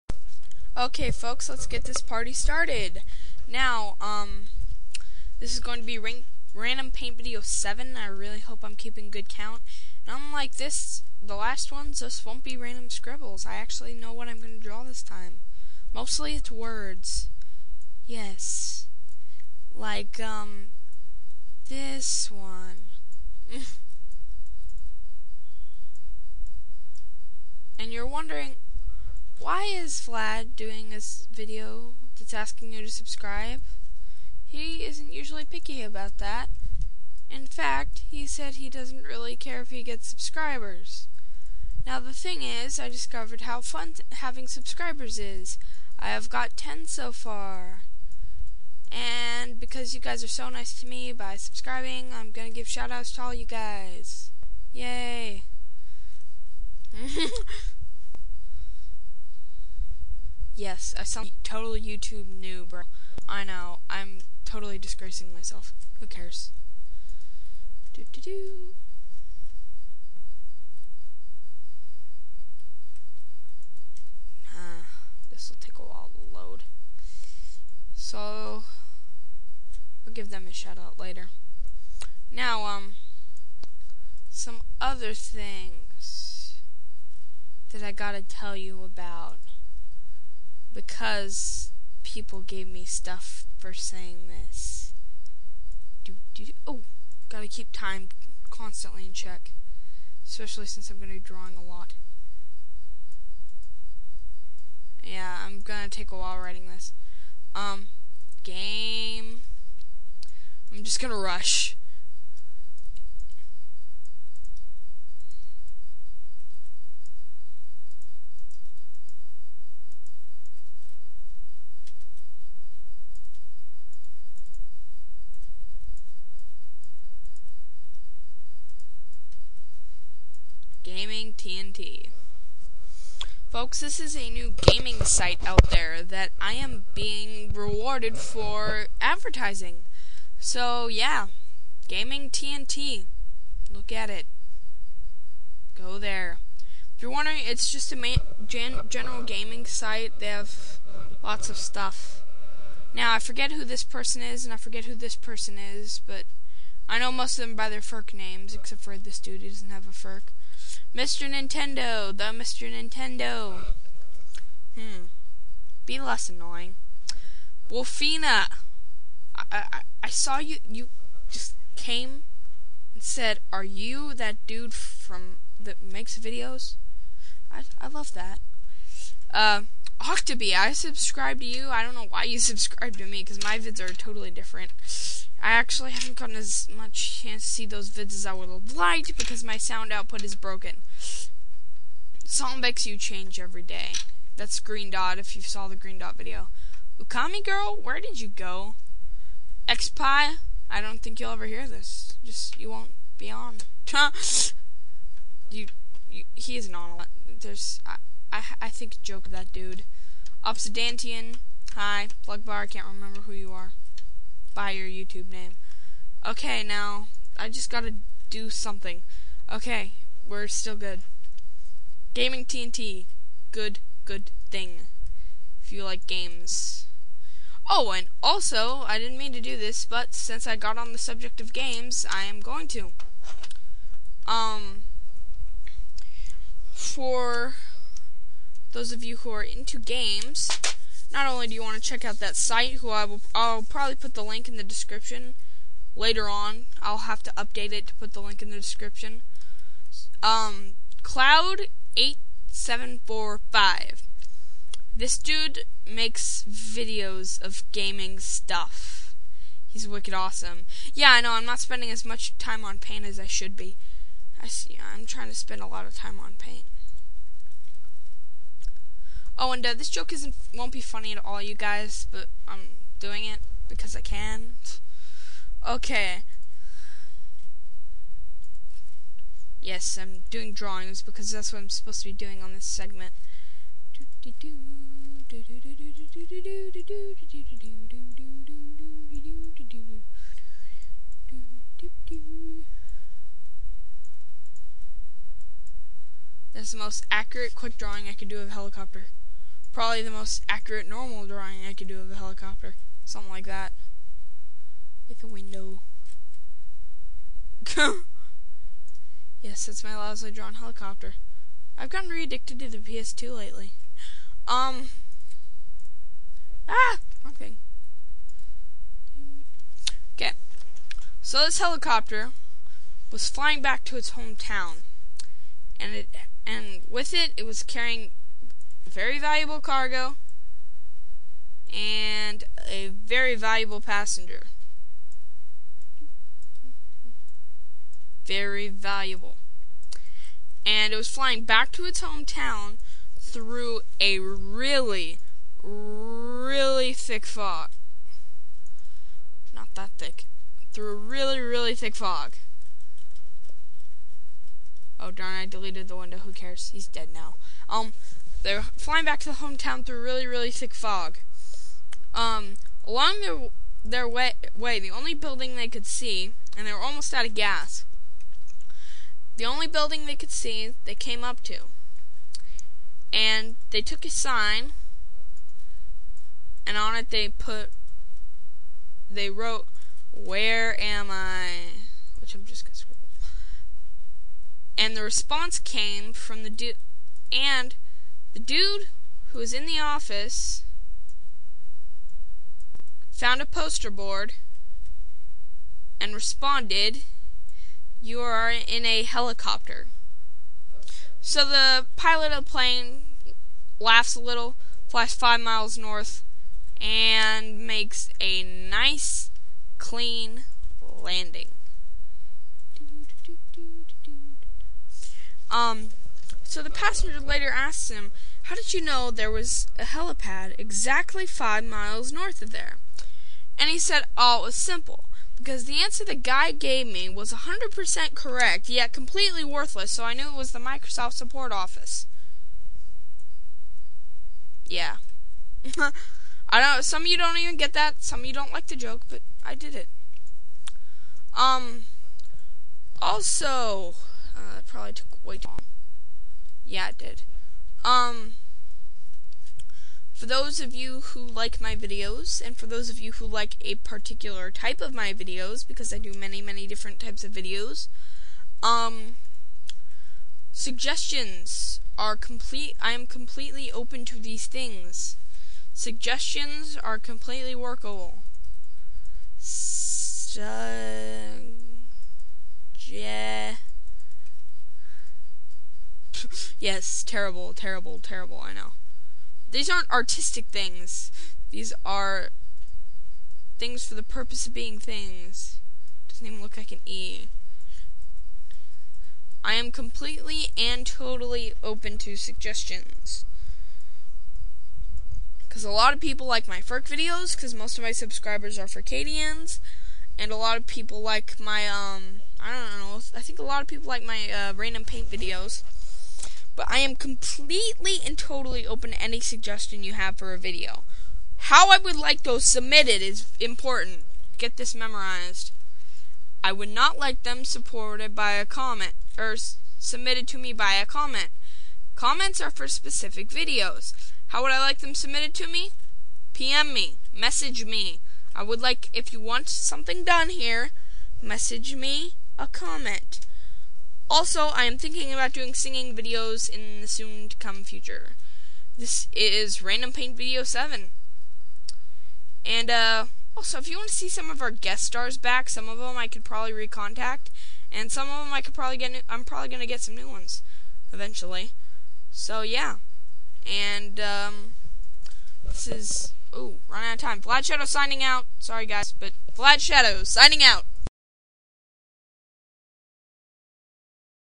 Random Paint Video 7: The Morning Announcements
Me talking about Gaming TnT, Cloud, My subscribers, and the beggining of my rather nOObish i-want-subscriptions binge.